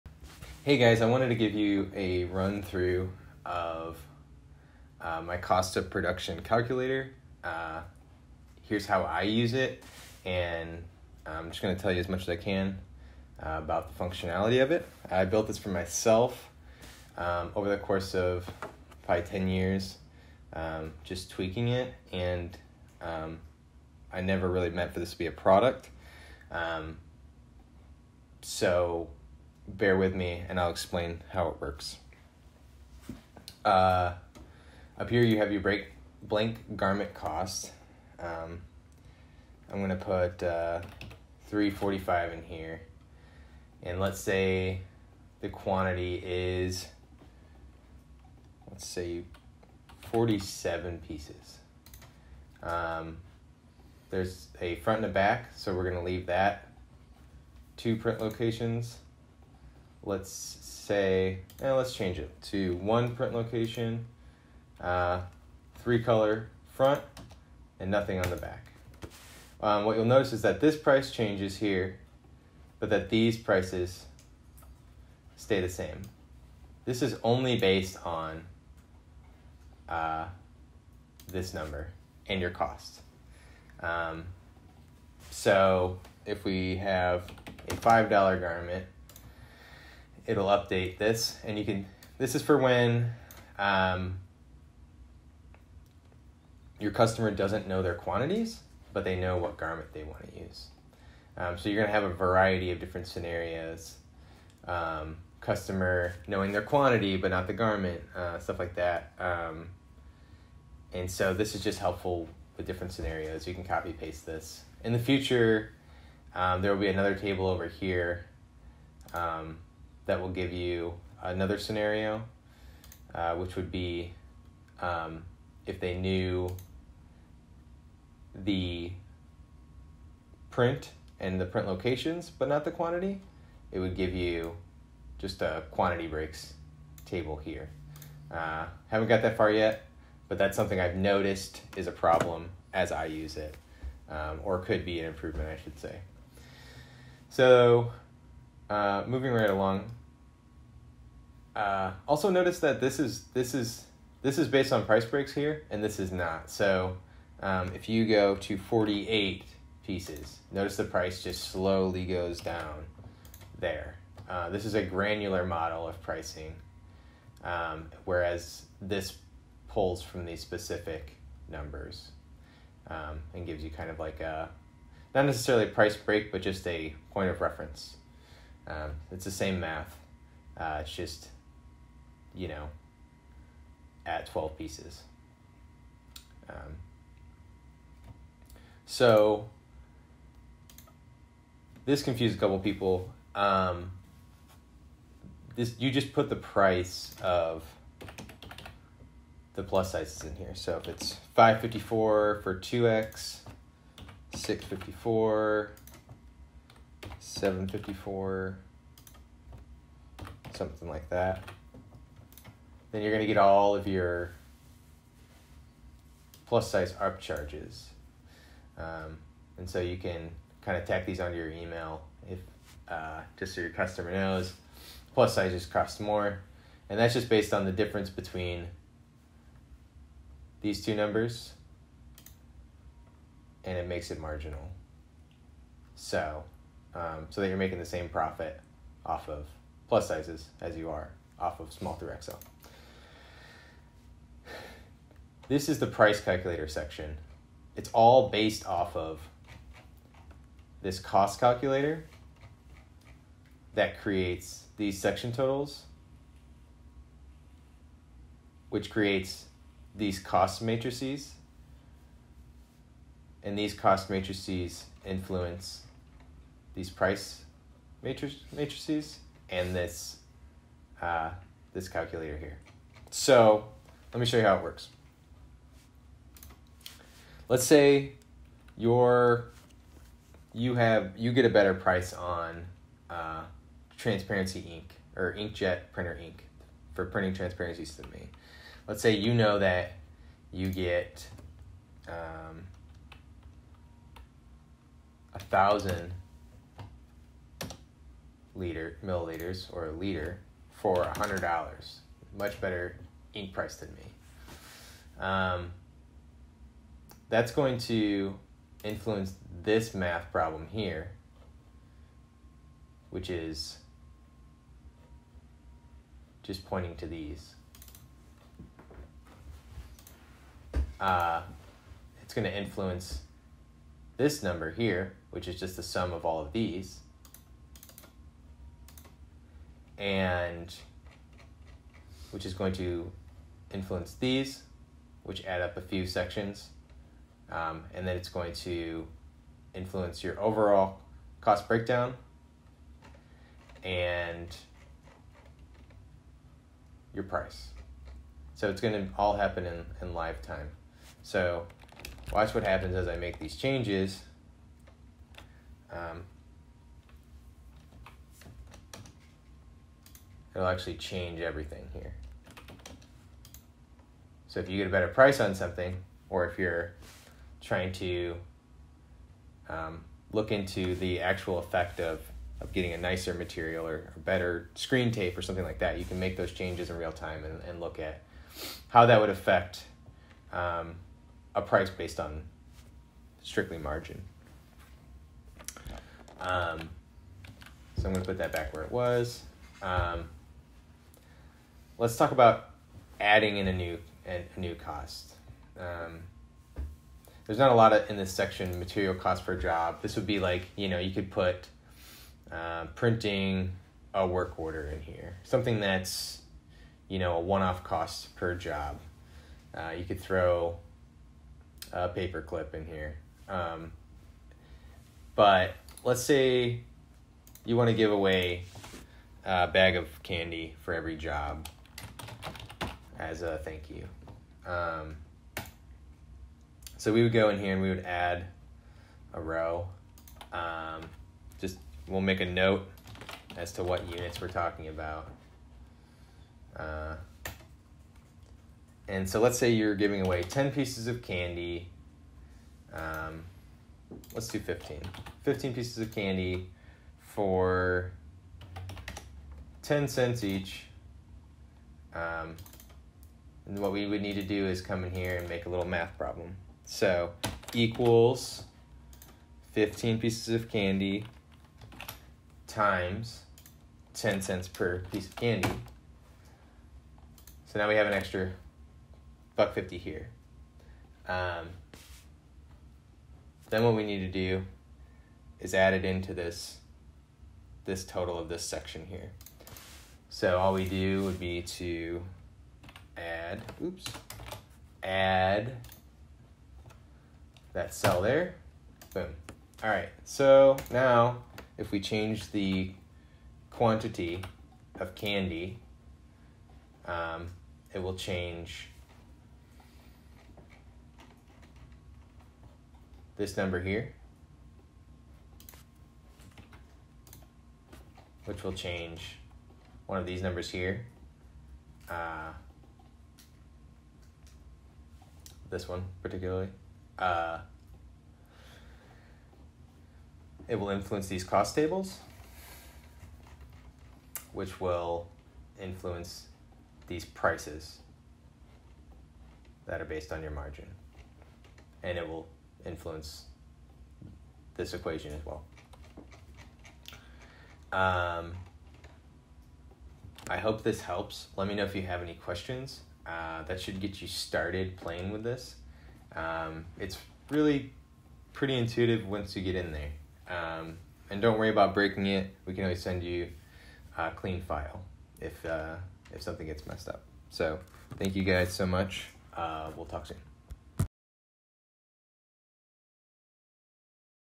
"Legendary Screen Printing Cost Calculator" - Walkthrough
A tutorial on getting the most out of Austin Caldwell's "Legendary Screen Printing Cost Calculator" Excel spreadsheet.